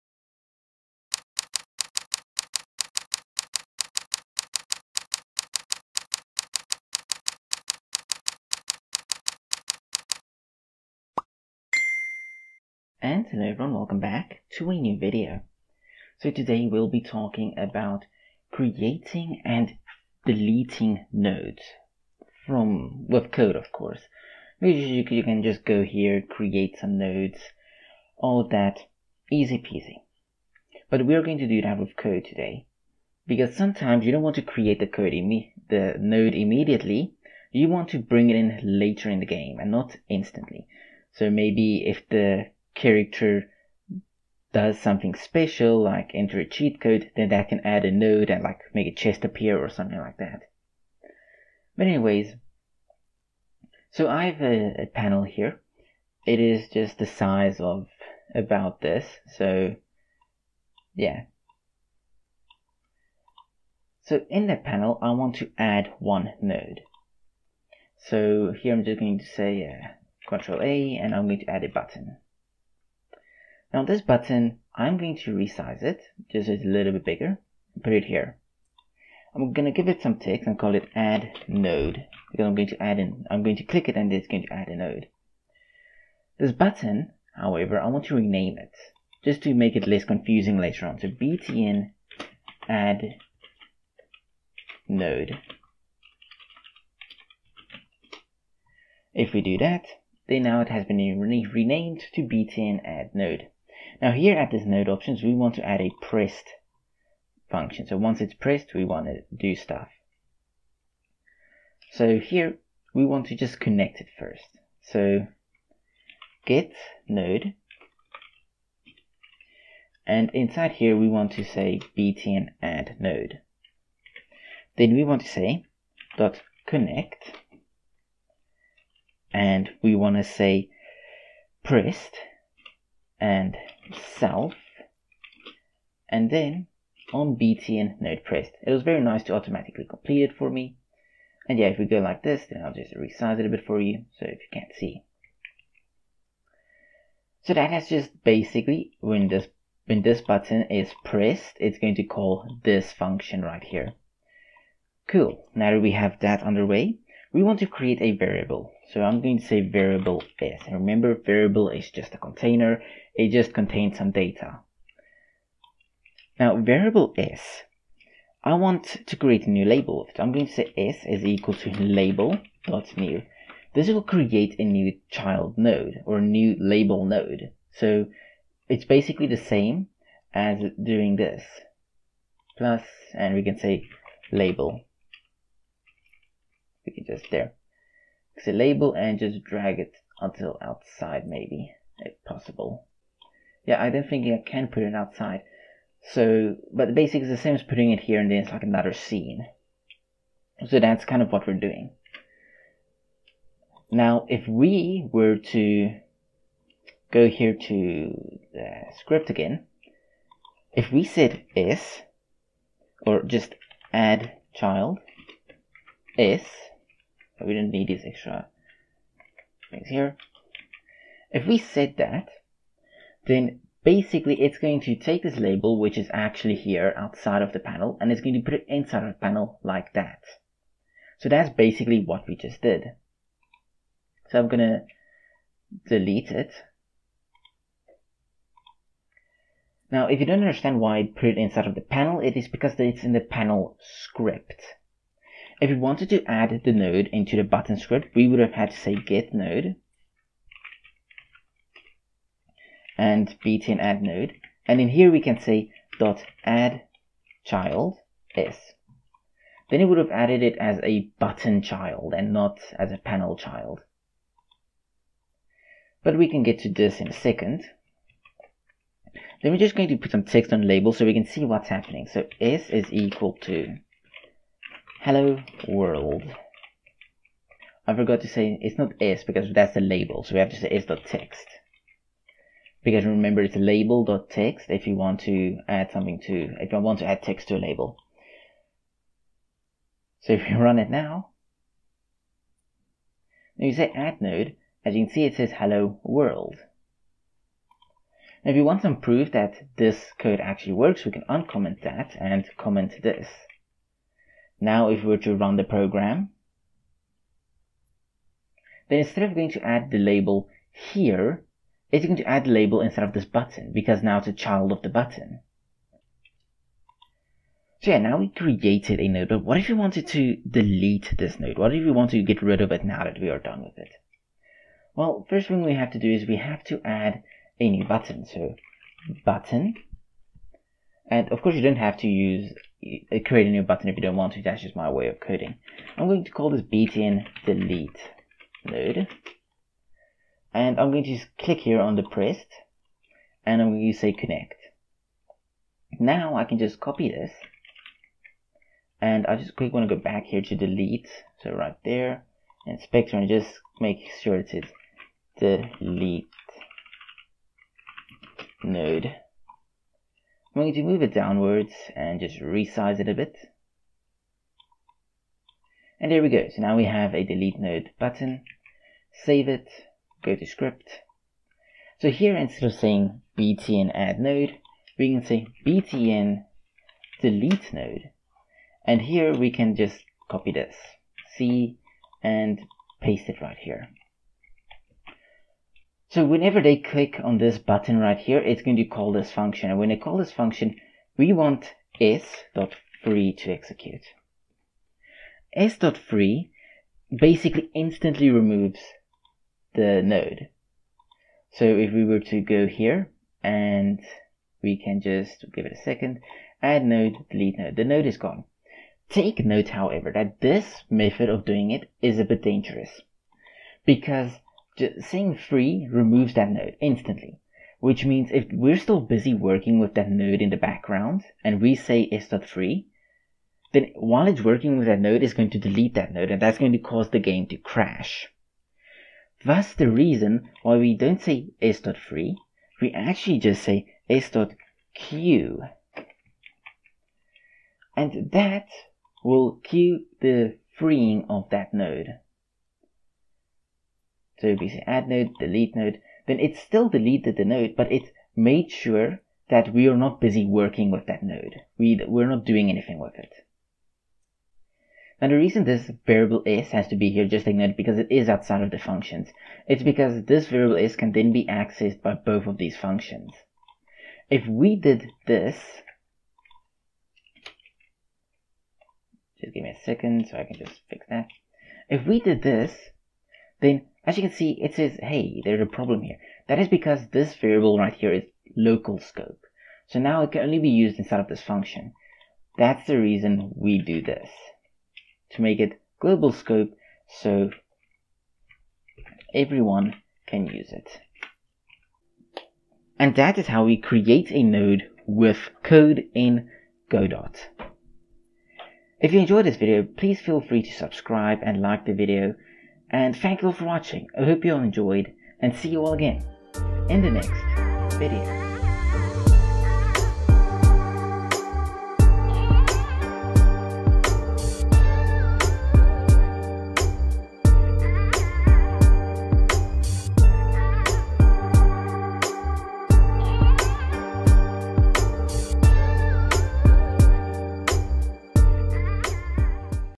Creating and deleting nodes - Godot Tutorial (Part 9)
Learn how to create nodes in Godot with code (GDScript). Sometimes you want to add a node to your game while it's running or if the user does something specific, this method can make your workspace look cleaner as well.